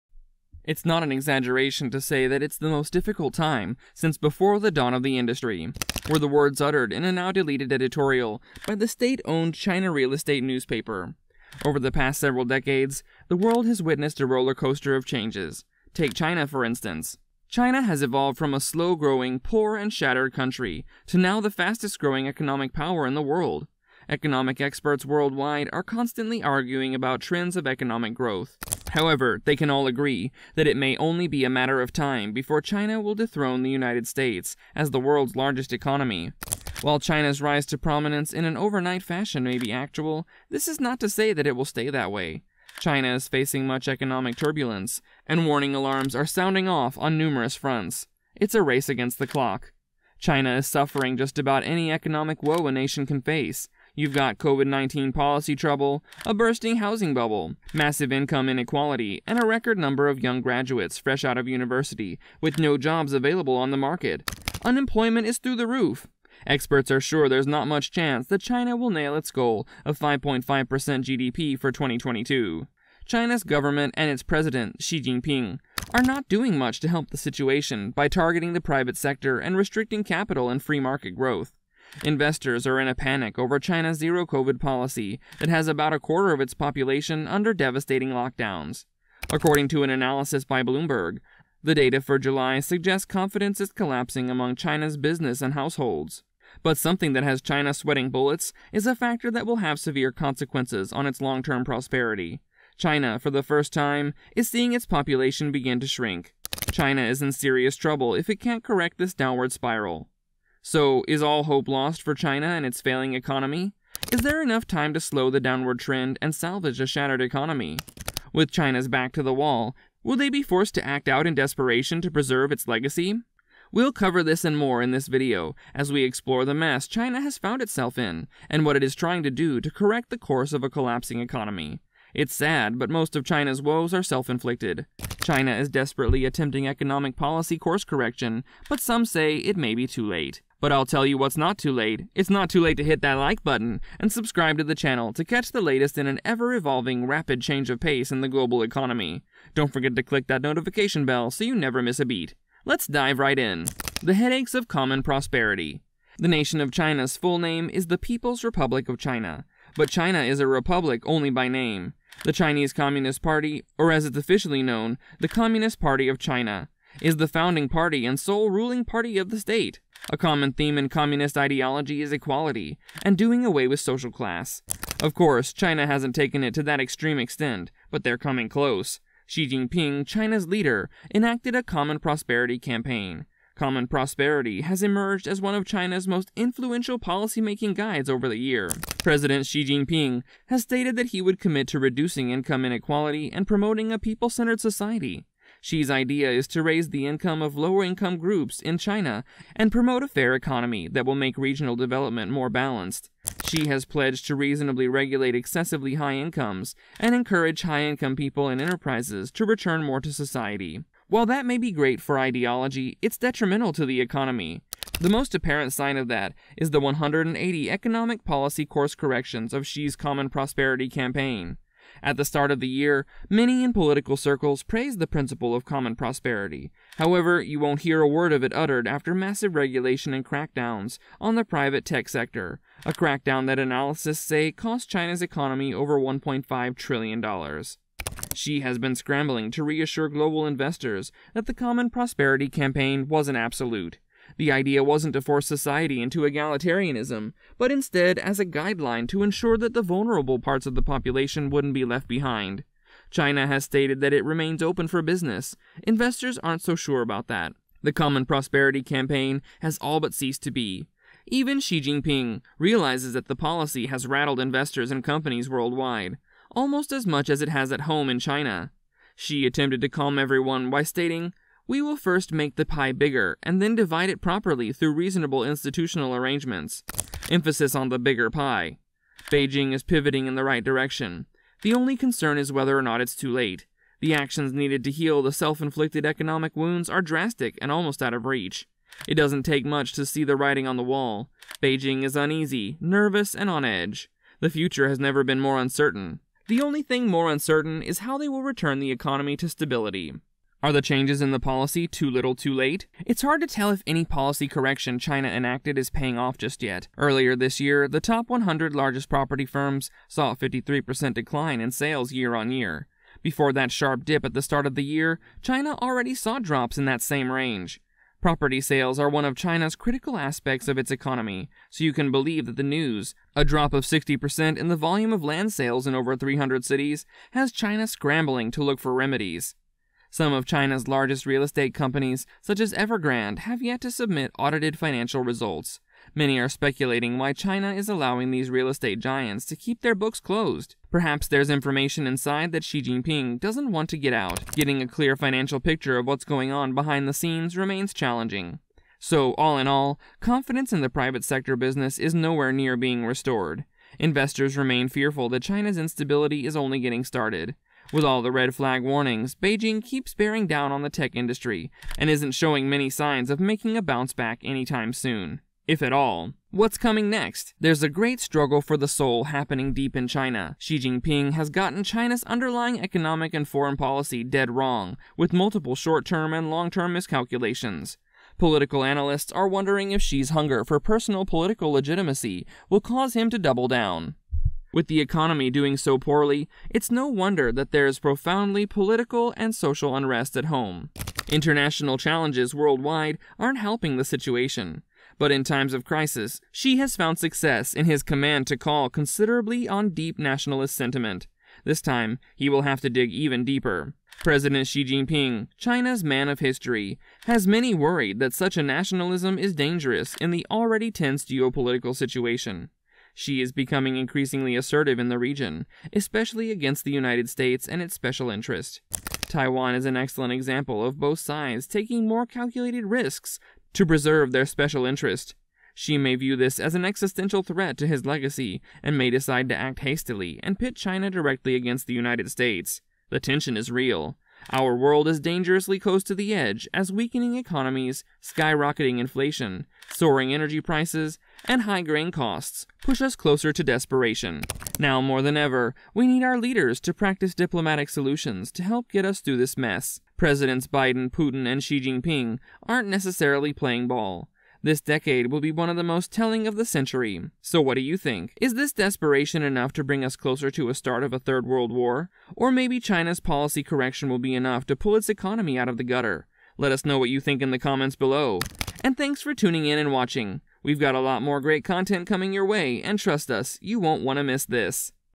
China’s Economy is Racing Against Time ( Reports Confirm )
China’s Economy is Racing Against Time – is it too late?
“It is not an exaggeration to say it is the most difficult time since before the dawn of the industry,” were the words uttered in a now-deleted editorial by the state-owned China Real Estate Newspaper.

Over the past several decades, the world has witnessed a roller coaster of changes. Take China, for instance. China has evolved from a slow-growing, poor, and shattered country to now the fastest-growing economic power in the world.

Economic experts worldwide are constantly arguing about trends of economic growth. However, they all can agree that it may only be a matter of time before China will dethrone the United States as the world’s largest economy.

While China’s rise to prominence in an overnight fashion may be actual, that is not to say that it will stay that way. China is facing much economic turbulence, and warning alarms are sounding off on numerous fronts.

It’s a race against the clock.

China is suffering just about any economic woe a nation can face. You’ve got Covid-19 policy trouble, a bursting housing bubble, massive income inequality, and a record number of young graduates fresh out of university with no jobs available on the market – unemployment is through the roof. Experts are sure there’s not much chance that China will nail its goal of 5.5% GDP for 2022.

China’s government and its president, Xi Jinping, are not doing much to help the situation by targeting the private sector and restricting capital and free market growth. Investors are in a panic over China’s zero-Covid policy that has about a quarter of its population under devastating lockdowns. According to an analysis by Bloomberg, the “data for July suggest confidence is collapsing among China’s business and households.”

But something that has China sweating bullets is a factor that will have severe consequences on its long-term prosperity – China, for the first time, is seeing its population begin to shrink. China is in serious trouble if it can’t correct this downward spiral.

So is all hope lost for China and its failing economy?

Is there enough time to slow the downward trend and salvage a shattered economy?